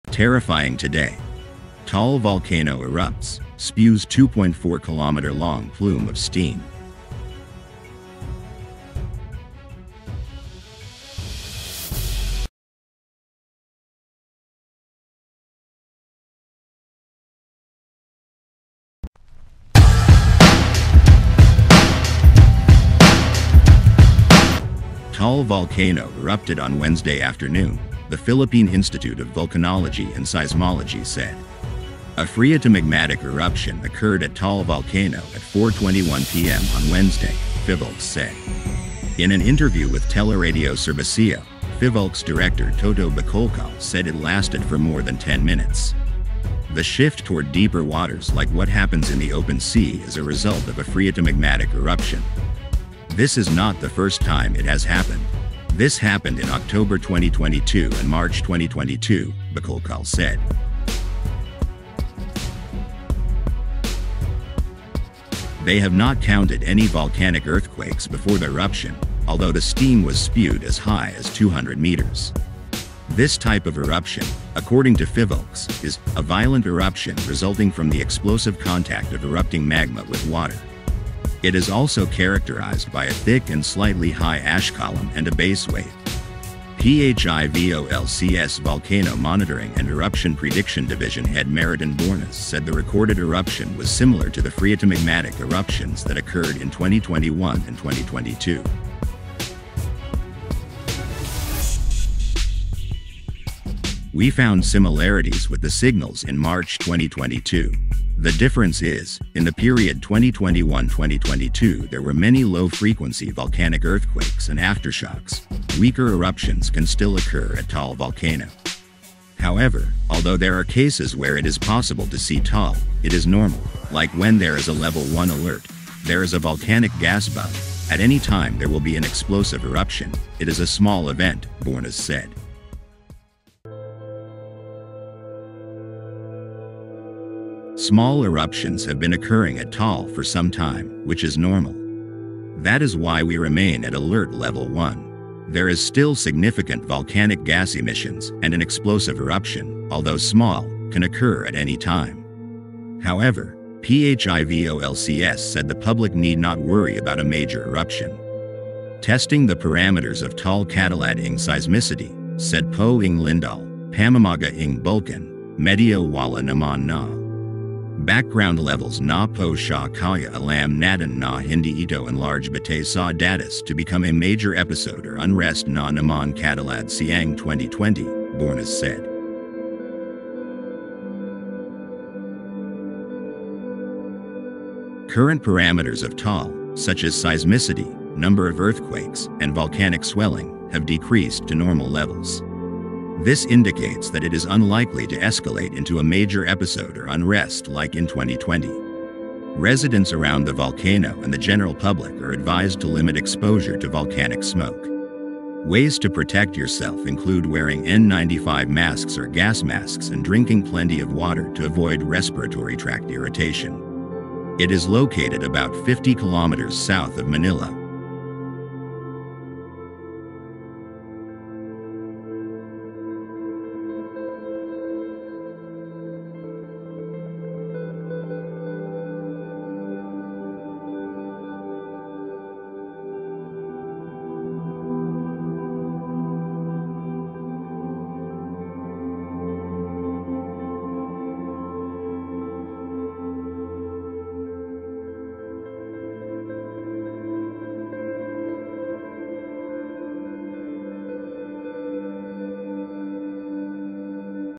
You Won't Believe the POWERFUL Eruption of Taal Volcano in Philippines
Taal volcano erupted on Wednesday afternoon, the Philippine Institute of Volcanology and Seismology said. A phreatomagmatic eruption occurred at Taal Volcano at 4:21 p.m. on Wednesday, Phivolcs said. In an interview with Teleradyo Serbisyo, Phivolcs director Toto Bacolcol said it lasted for more than 10 minutes. The shift toward deeper waters like what happens in the open sea is a result of a phreatomagmatic eruption. This is not the first time it has happened. This happened in October 2022 and March 2022," Bacolcol said.

They have not counted any volcanic earthquakes before the eruption, although the steam was spewed as high as 200 meters. This type of eruption, according to Phivolcs, is "a violent eruption resulting from the explosive contact of erupting magma with water. It is also characterized by a thick and slightly high ash column and a base wave. PHIVOLCS Volcano Monitoring and Eruption Prediction Division Head Mariton Bornas said the recorded eruption is similar to the phreatomagmatic eruptions that occurred in 2021 and 2022.